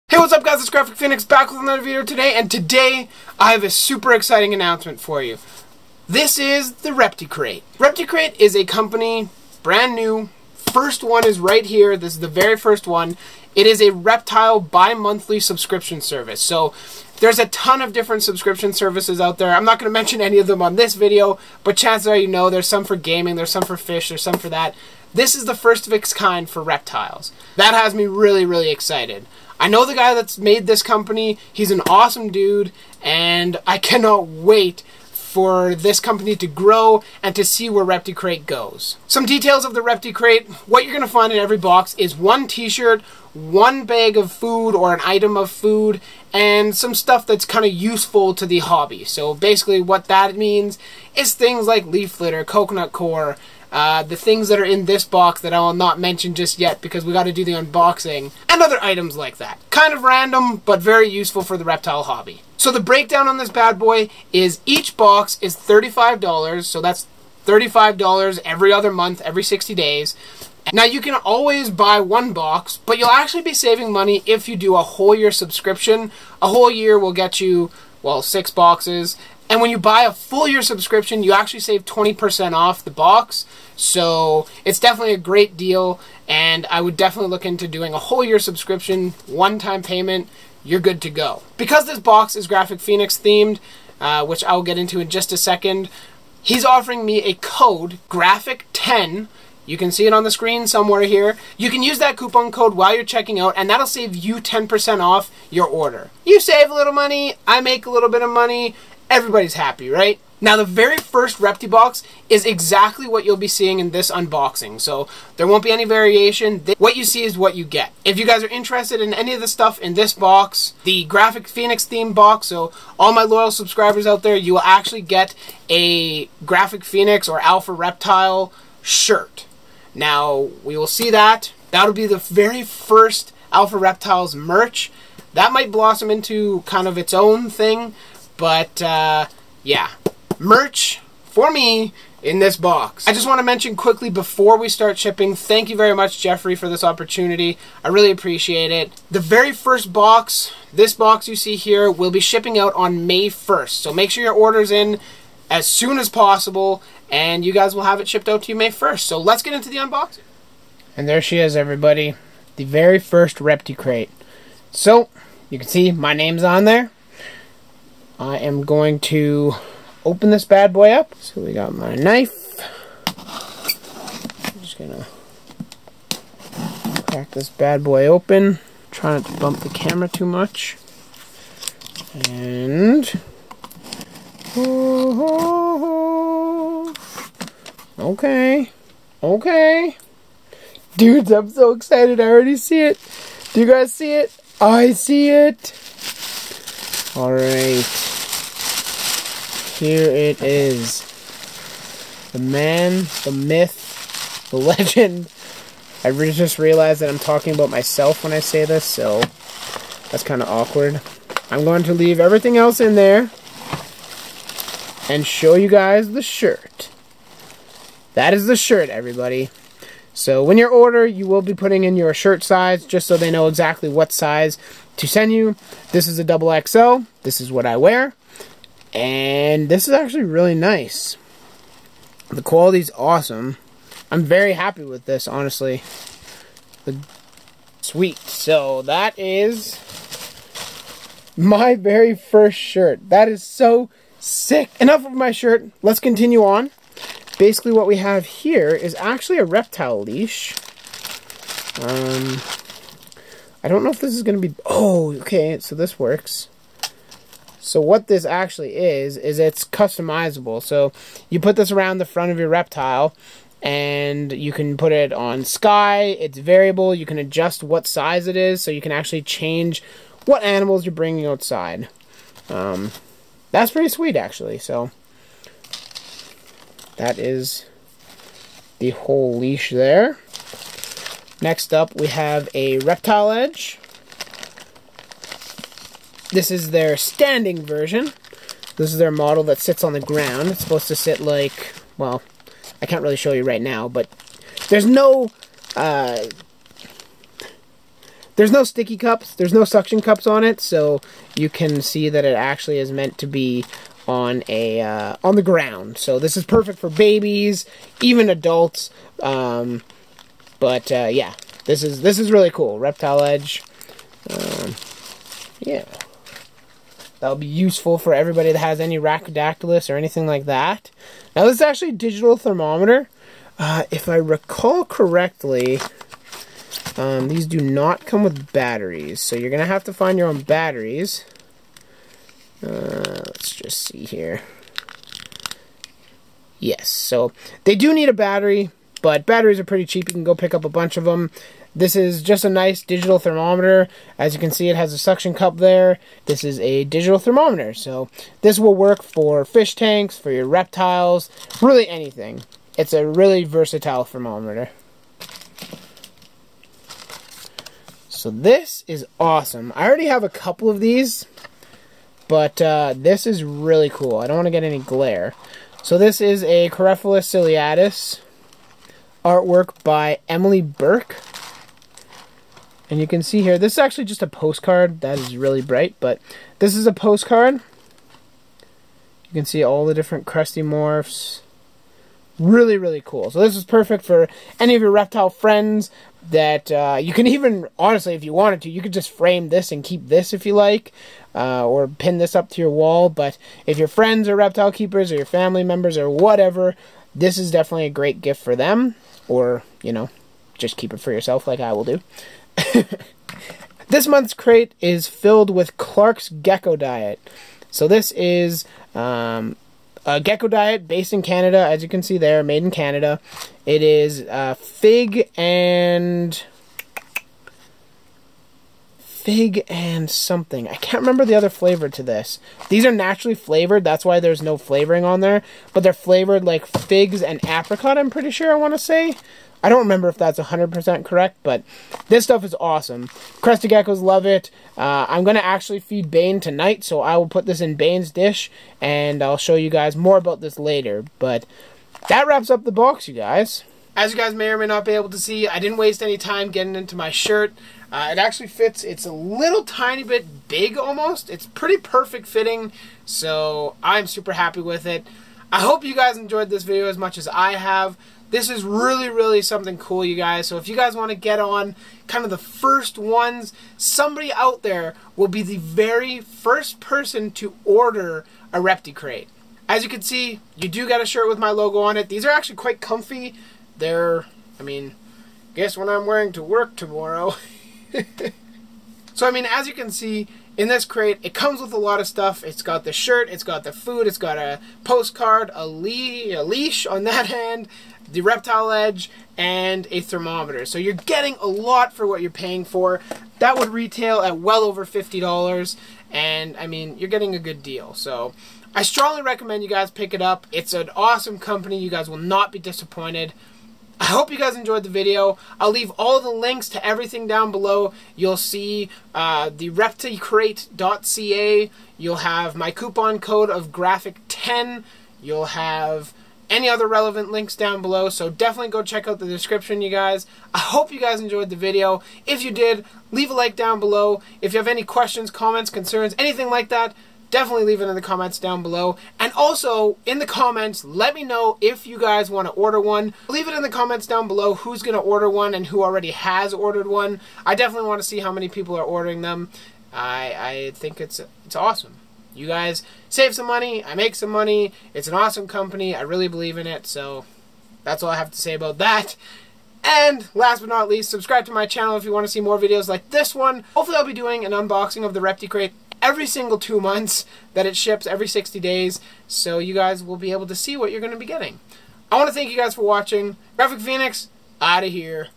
Unboxing the ReptiCrate -Reptile Subscription Box
Brand new Reptile subscription Box, Pick up your box today! :)